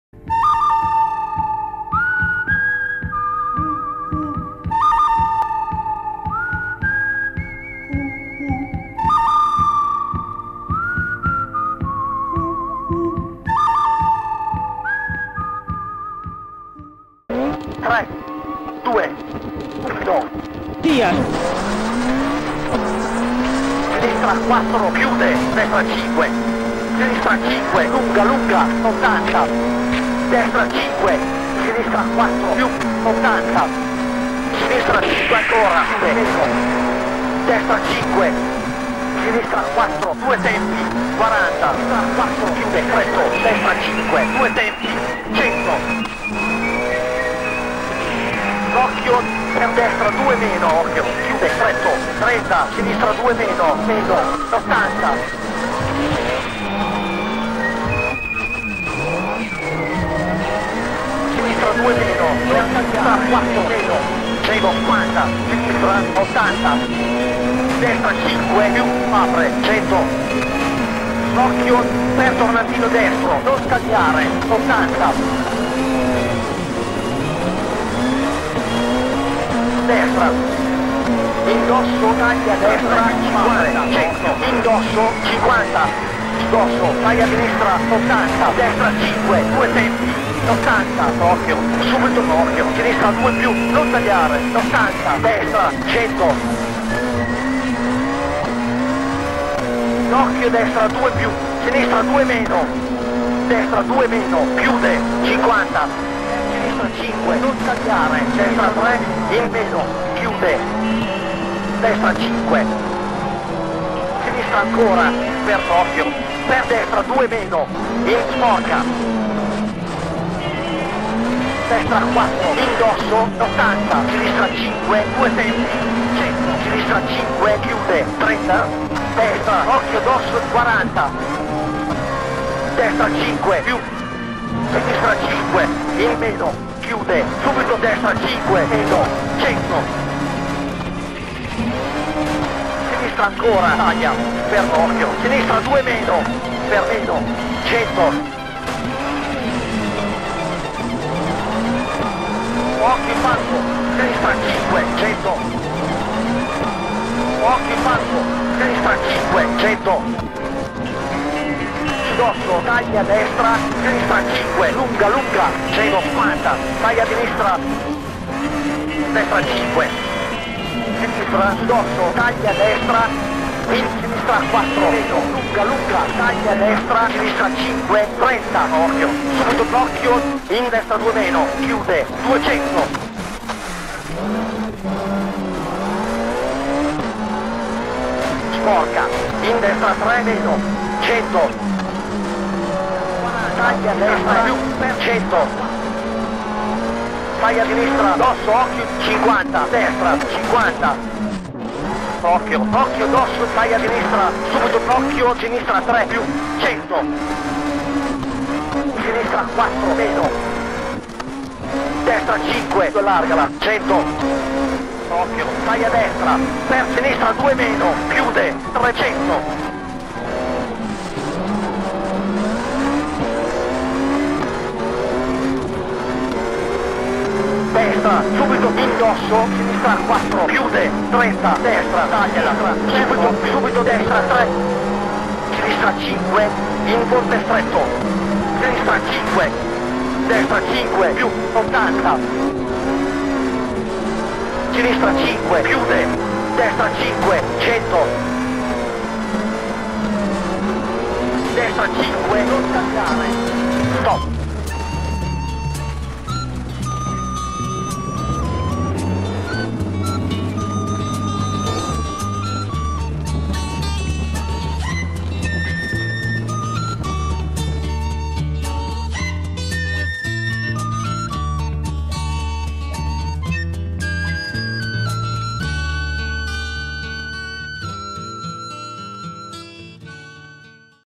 RealRally Clio s1600 @ Sherwood Forest II Summer (nuova PS 2018)
Divertiamoci un po' con un video scherzoso che riassume un po le mie impressioni guidando su questa nuova PS 2018, molto impegnativa